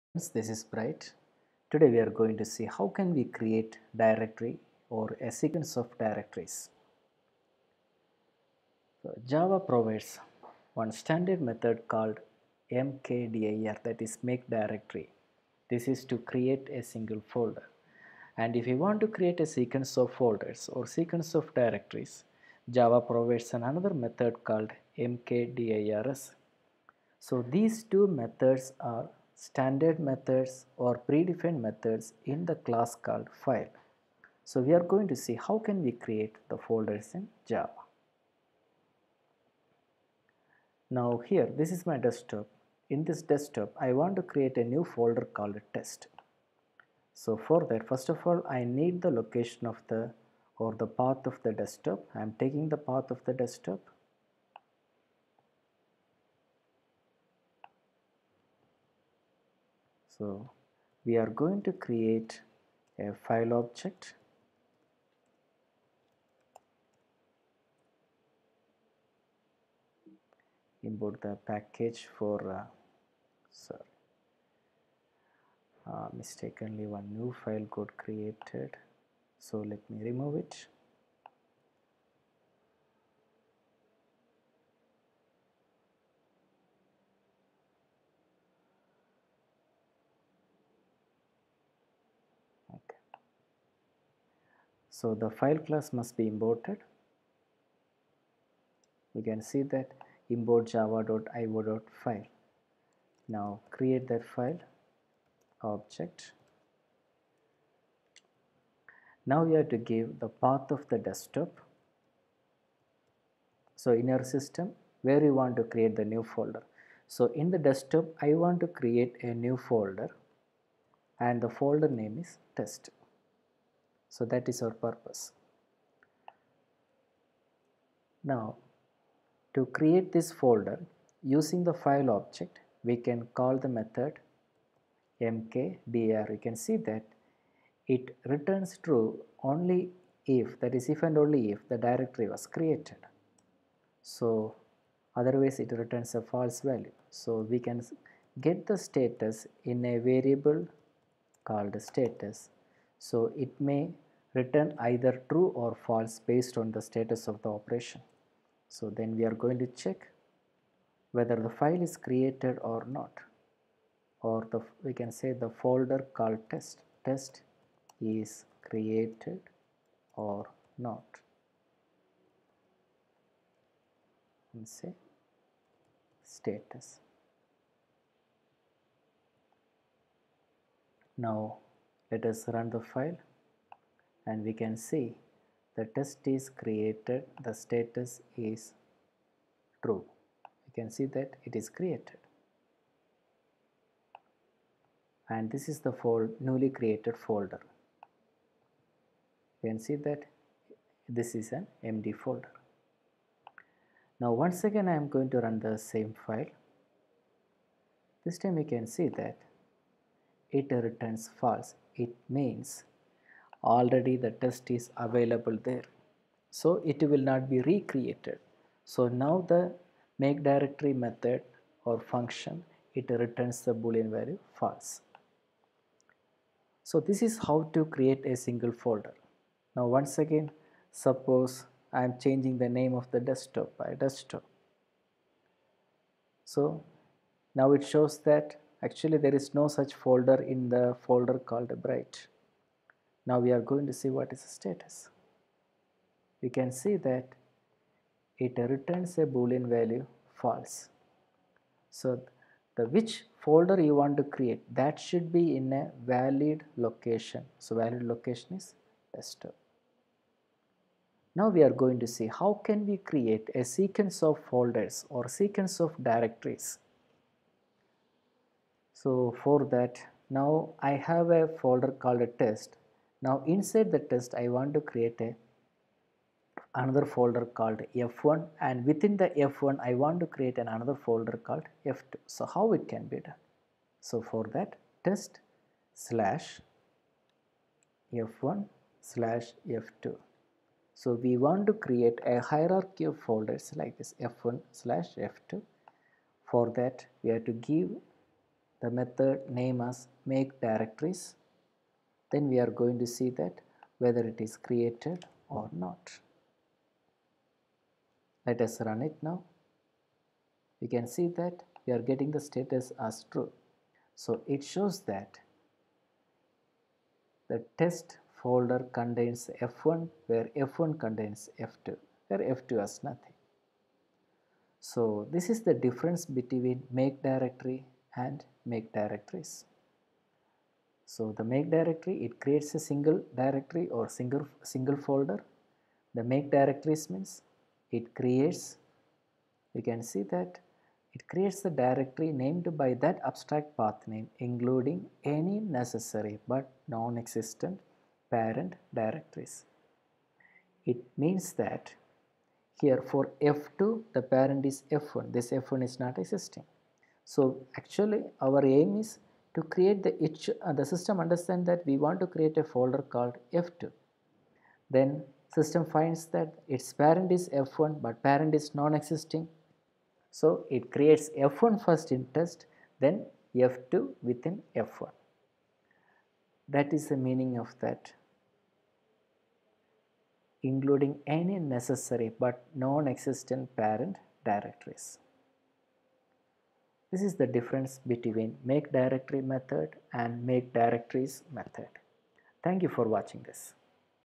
Demonstration of mkdir( ) and mkdirs( ) in Java
This demonstrates how to create folder or directory by using Java.